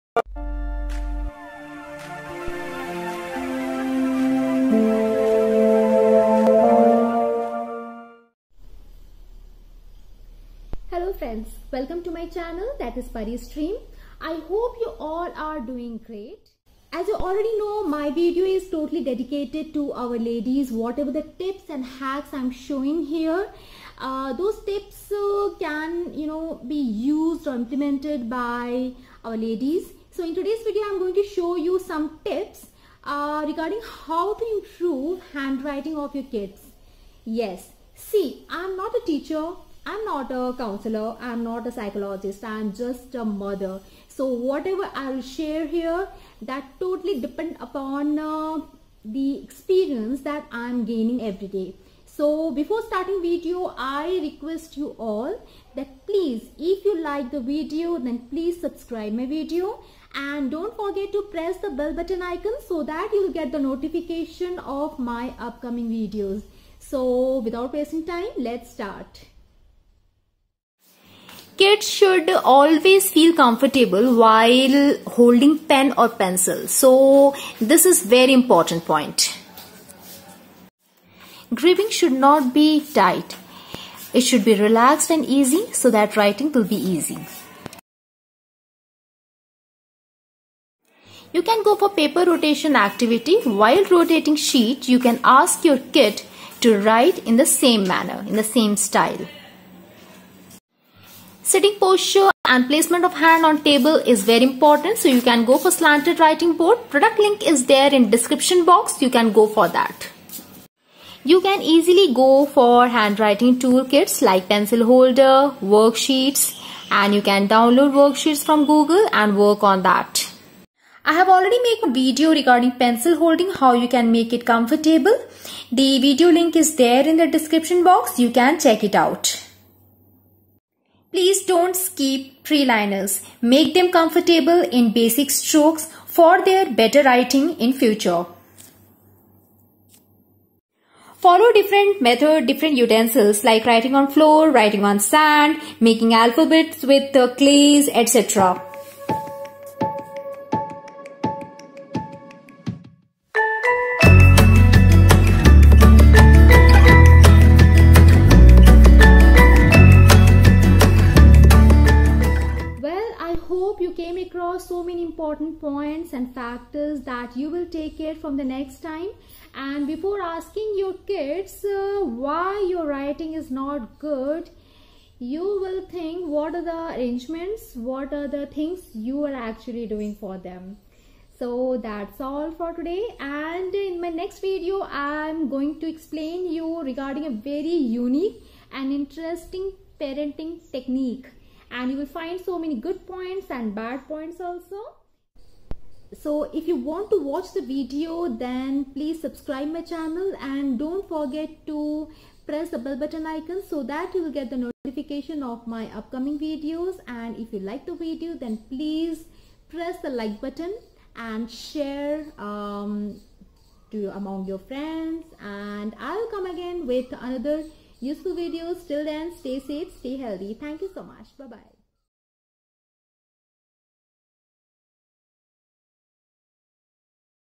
Handwriting Improvement Tips parisstream,writing improvement,how to improve handwriting
In this video,I am giving some tips ,how you can improve handwriting of your kids.Parents need to follow these steps if want your kids writing looks attractive.

Product Link for SLANTED BOARD

writing kaise sudhare
writing improvement in English
writing improvement tips
writing sudharne ke tarike
writing improvement